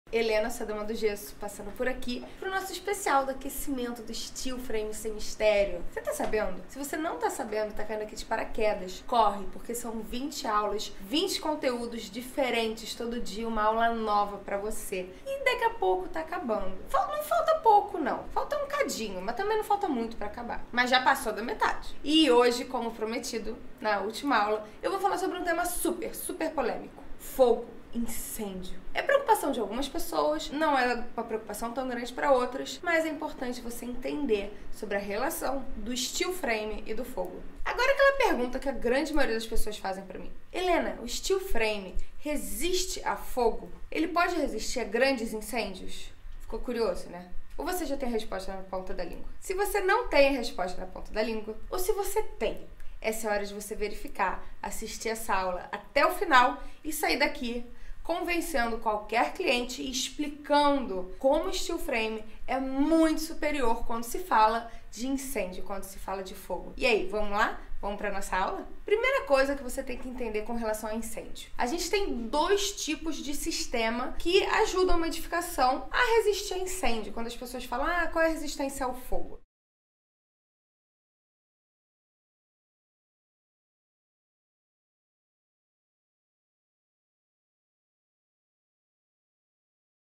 RESISTÊNCIA A FOGO: o Steel Frame pode resistir a incêndios?
Hoje você vai saber TUDO sobre um dos temas mais polêmicos que envolvem o Steel Frame! Preparado?

Você sabia?
O tempo que os materiais usados em uma obra de Steel Frame suportam, como as placas cimentícias, tem resistência superior a 120 minutos, o dobro indicado pelo Corpo de Bombeiros que regulam que os materiais usados em construções devem suportar pelo menos 60 minutos.